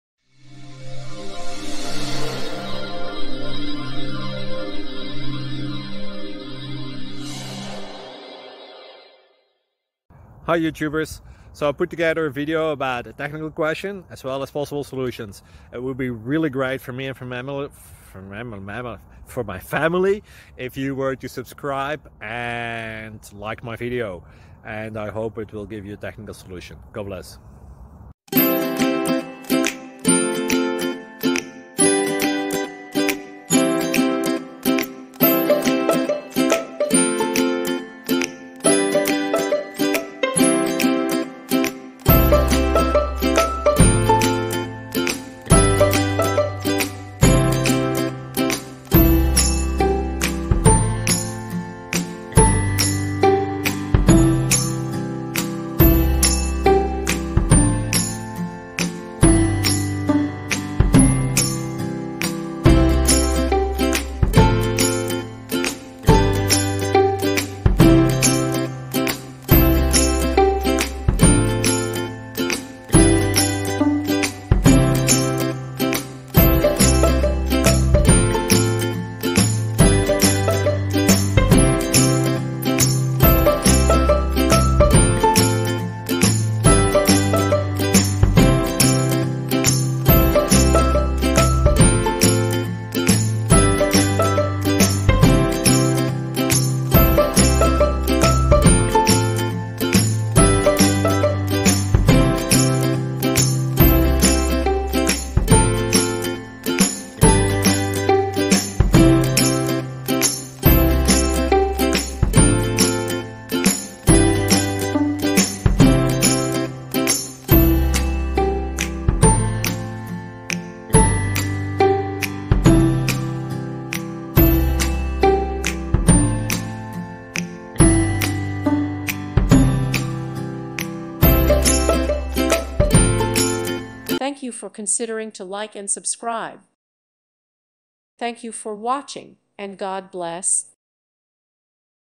Static analysis for finding capitalisation / case inconsistencies in file names
Software Engineering: Static analysis for finding capitalisation / case inconsistencies in file names

With thanks & praise to God, and with thanks to the many people who have made this project possible! You are responsible for your own actions.